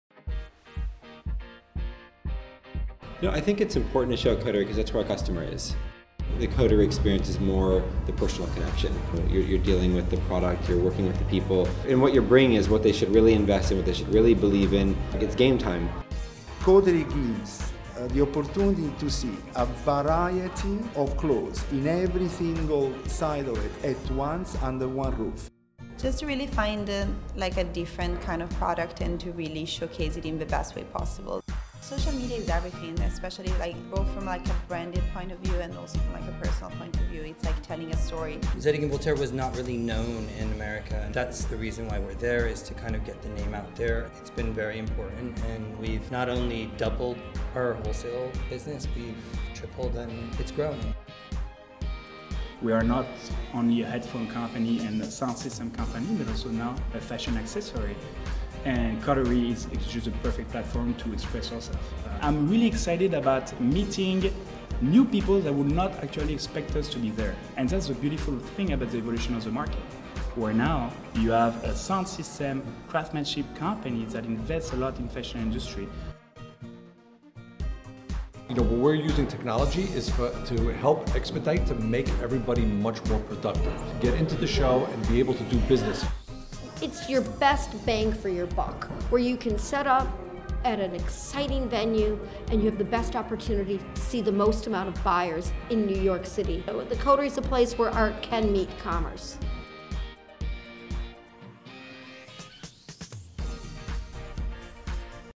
Coterie x Pryma NYFW
This past Thursday, New York Fashion Week officially passed on the torch to Coterie. Trade show producer UBM hosted a party at the World of McIntosh townhouse in SoHo. Attendees were granted access to preview collections belonging to 60 of the top brands showing at Coterie, a convenience for those who might typically miss the tradeshow due to demanding travel schedules.